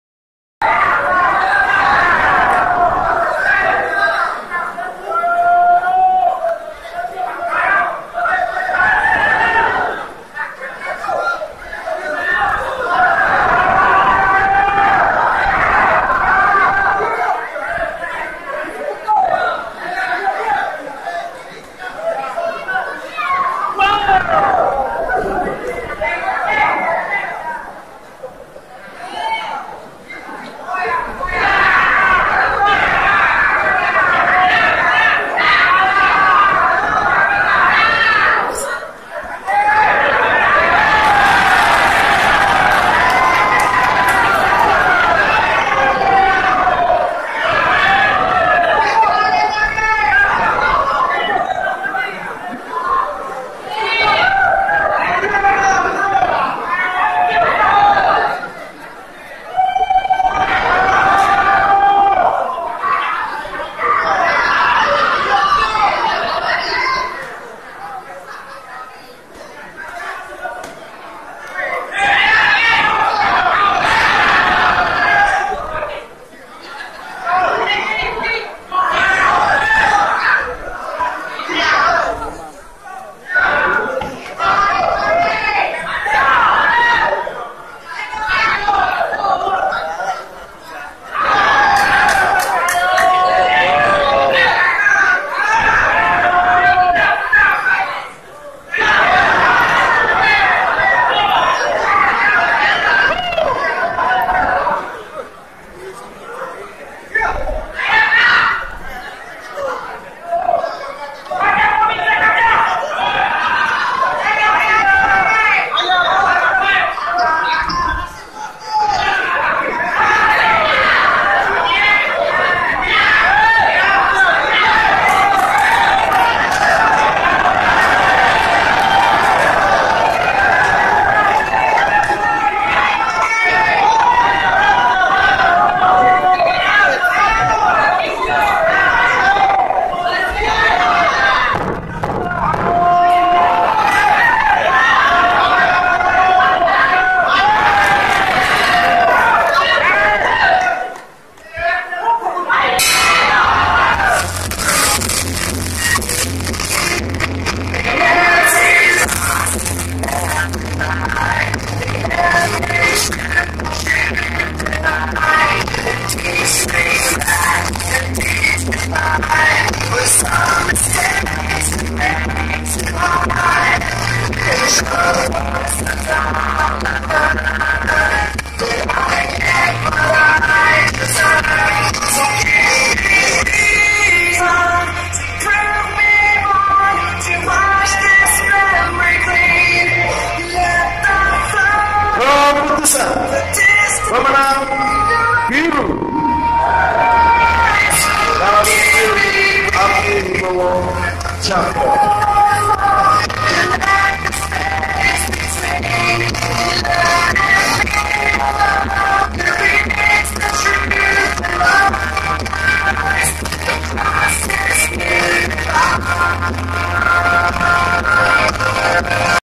Pertandingan Tinju Kapolda Cup 2018 di Balai Sarbini, Jakarta - Kodam Jaya
Memperebutkan Piala Kapolda Metro Jaya, Irjen Polisi Idham Azis diikuti petinju binaan dari TNI